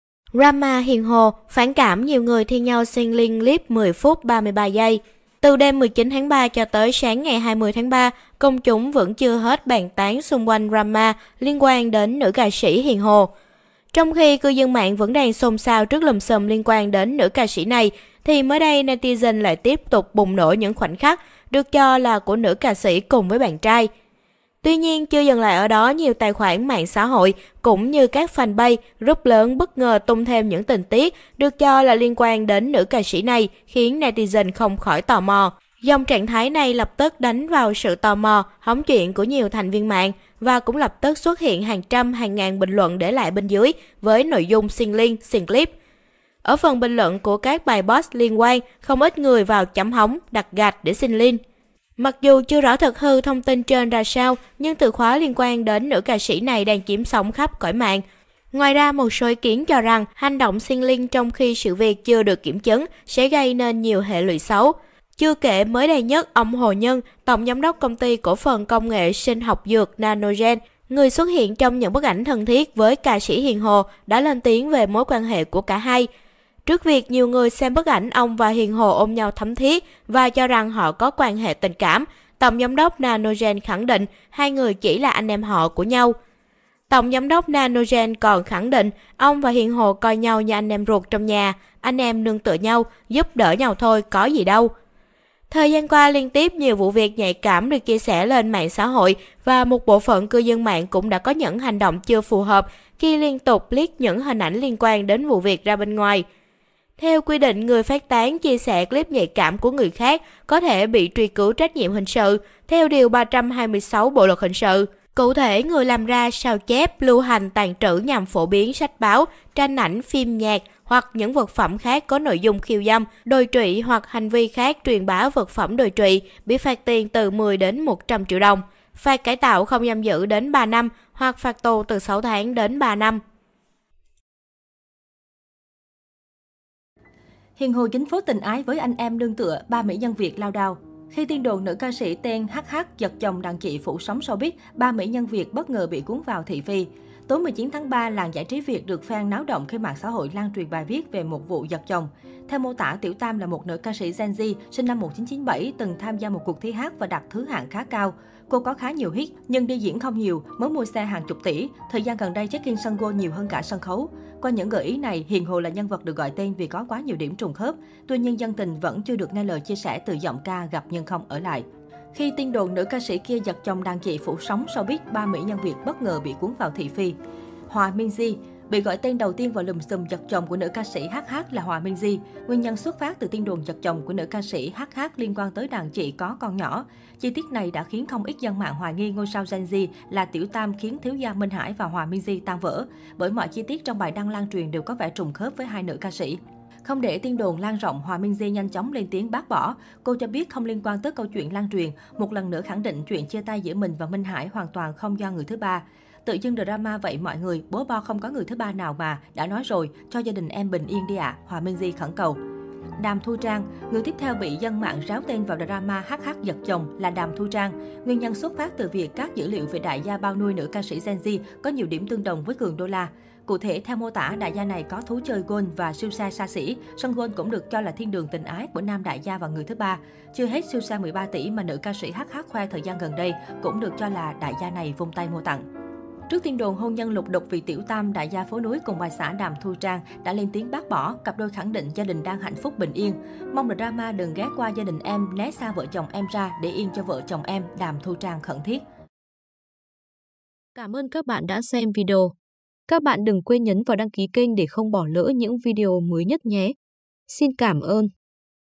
Clip 10 phút 33 giây - Drama U60 và Hiền Hồ
Thực hư clip 10 phút 33 giây, Hiền Hồ và drama với anh trai mưa U 60, nương tựa nhau đi chơi golf,đi chùa,đi khâch sạn